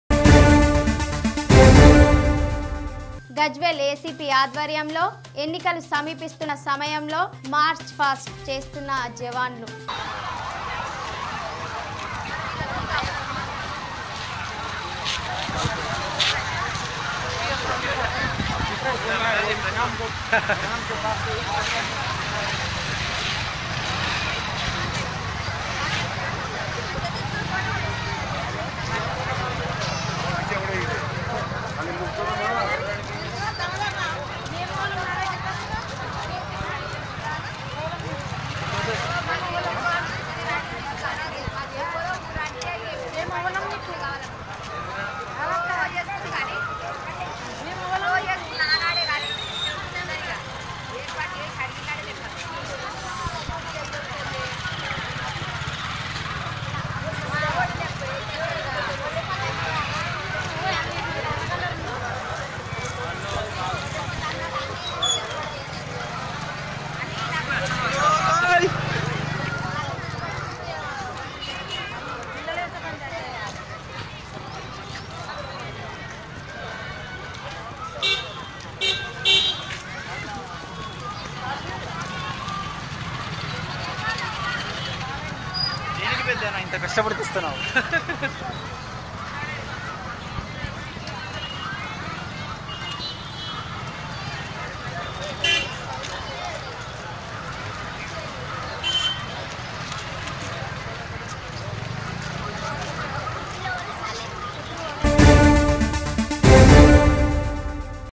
TS24 News గజ్వేల్ లో మార్చ్ పాస్ట్ నిర్వహించిన ఆర్మీ జవానులు
లో మార్చ్ పాస్ట్ నిర్వహించిన ఆర్మీ జవానులు